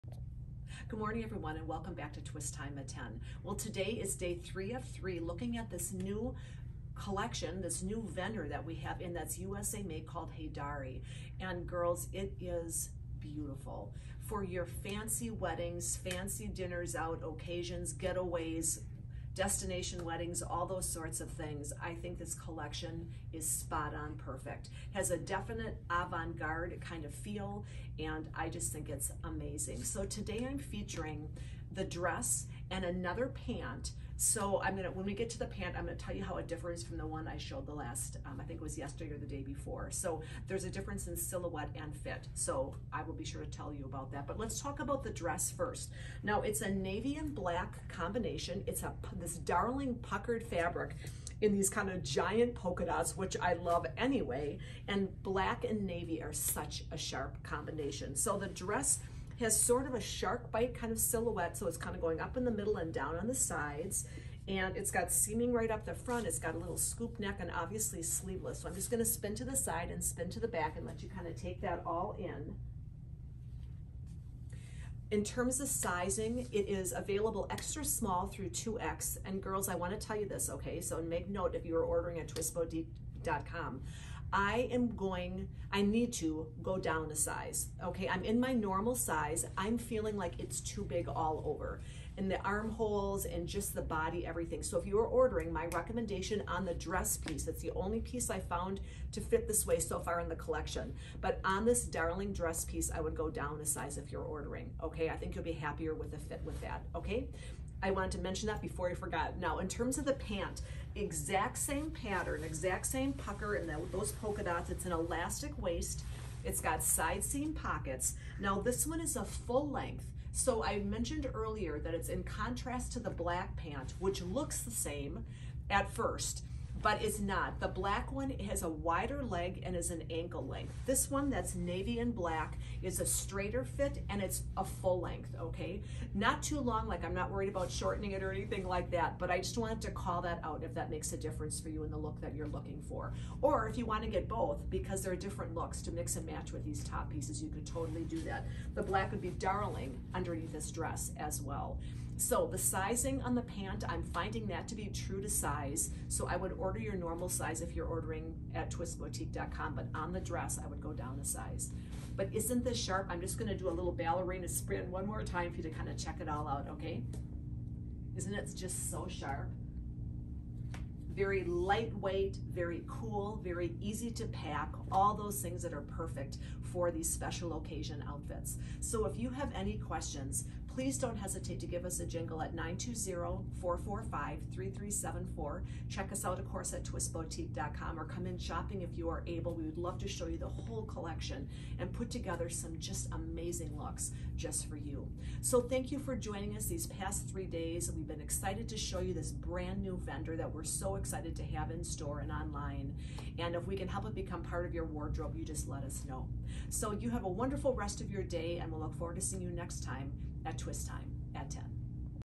HEYDARI Day 3
Day 3 of 3 showcasing Heydari- a brand new collection here at Twist Boutique that has an awesome Big City feel.  Today’s featured pieces are the navy/ black pucker textured dress with shark-bite hemline and the matching full-length pant.
XS-2X.  USA 🇺🇸 Made.